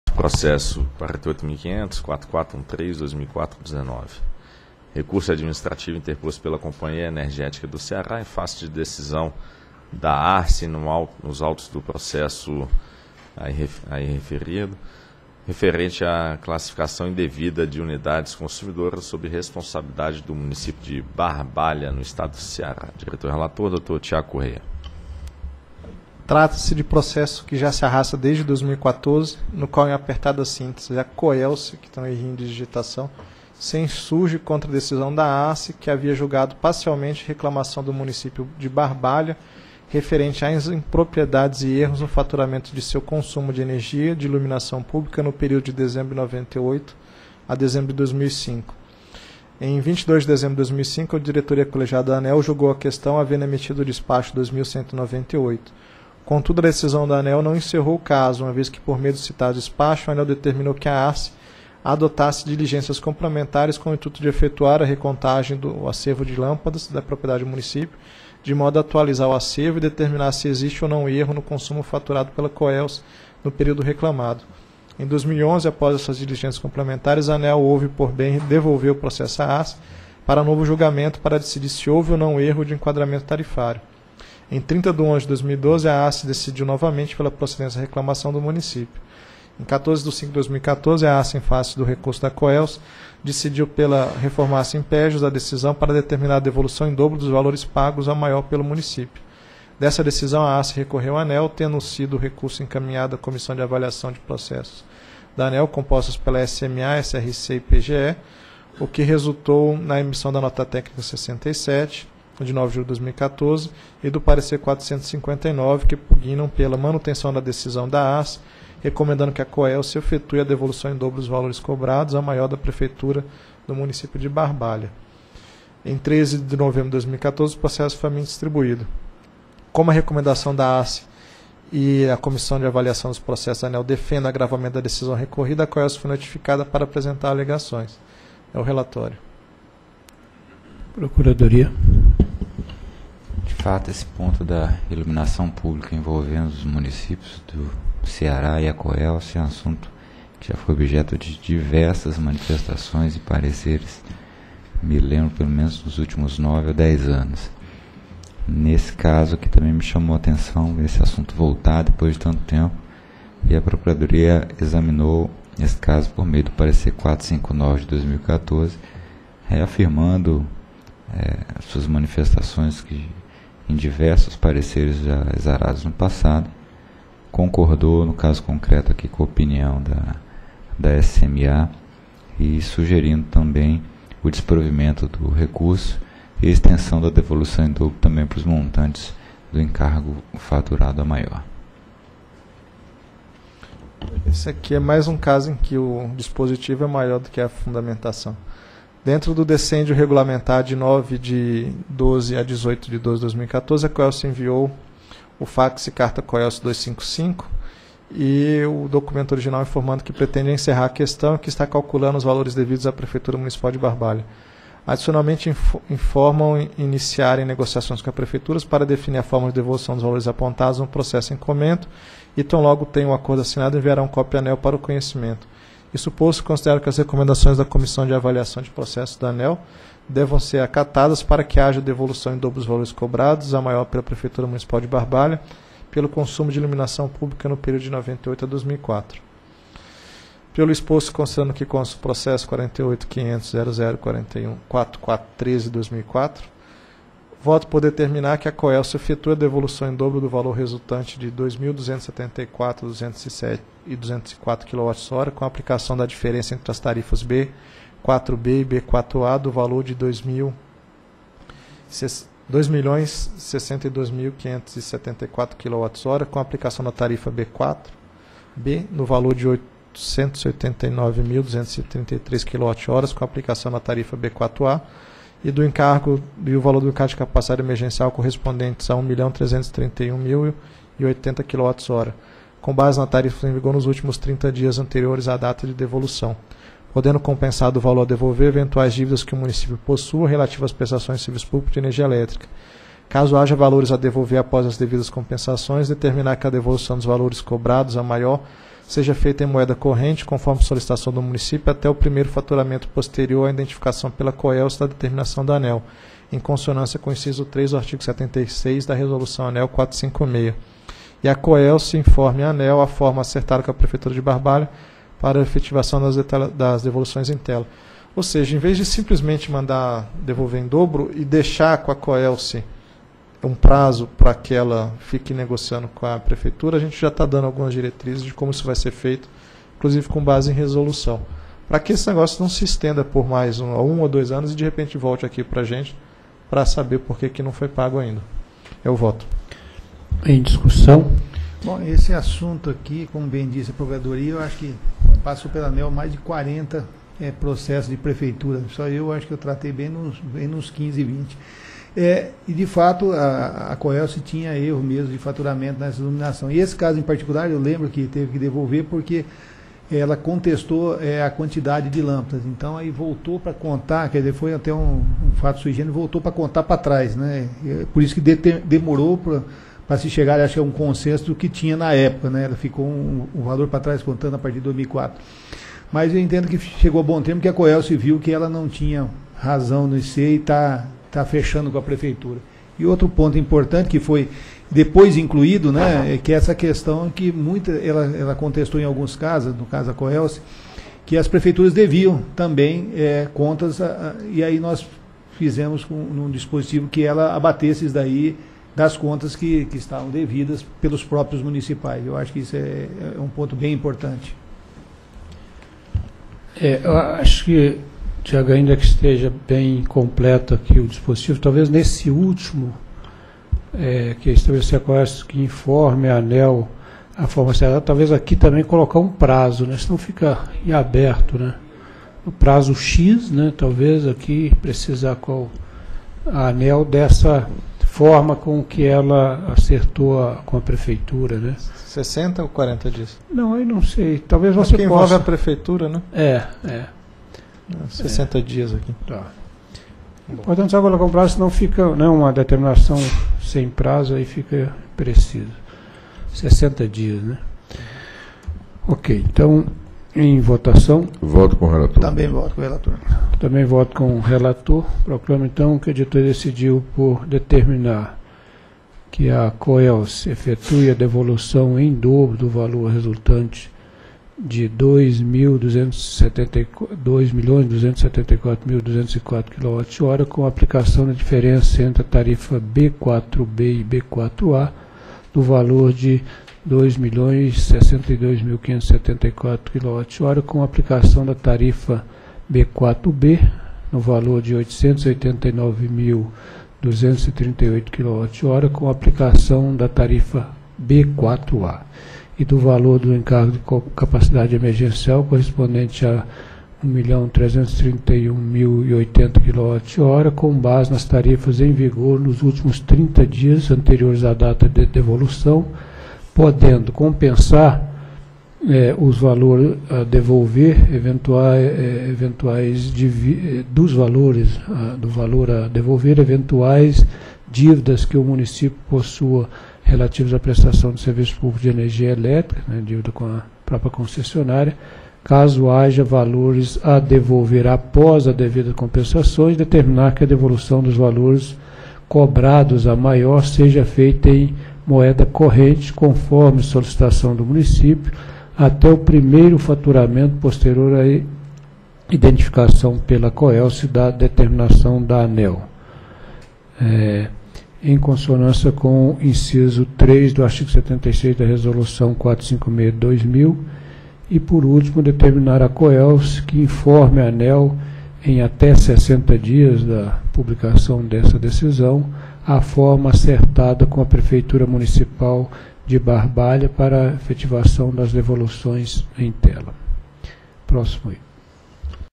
Item 4 - 4ª Reunião Pública Ordinária ANEEL 10/02/2015
Item 4. Processo: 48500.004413/2004-19. Assunto: Recurso Administrativo interposto pela Companhia Energética do Ceará – Coelce em face de decisão da Agência Reguladora dos Serviços Públicos Delegados do Estado do Ceará – ARCE nos autos do Processo PCEE/OUV/372/2003, referente à classificação indevida de unidades consumidoras sob responsabilidade do município de Barbalha, estado do Ceará. Área Responsável: Diretoria – DIR.
Relator: Diretor Tiago de Barros Correia.